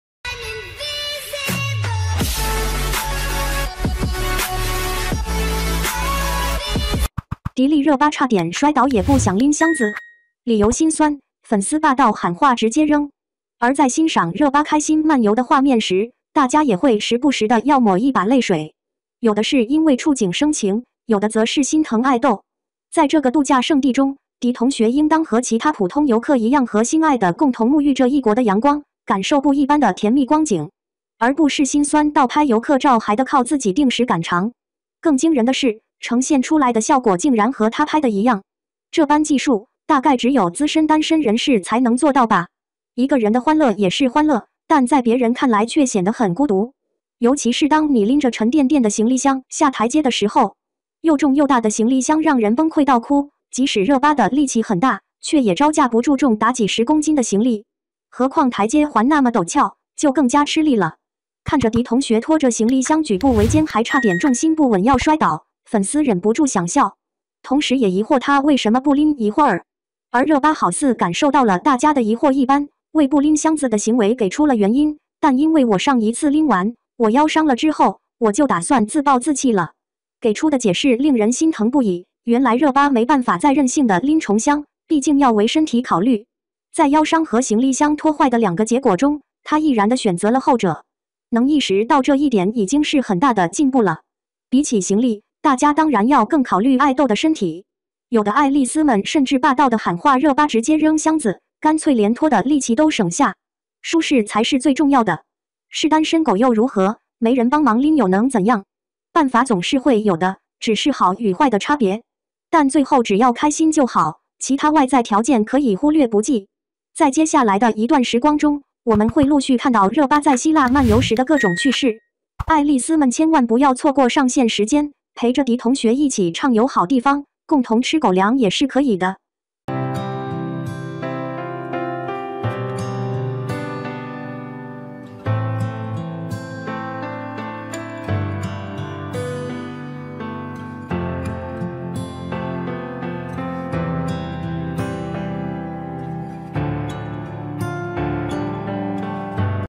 迪丽热巴 差点摔倒也不想拎箱子？理由心酸，粉丝霸道喊话直接扔
迪丽热巴差点摔倒也不想拎箱子？理由心酸，粉丝霸道喊话直接扔 | 明星新闻 MKN Star News.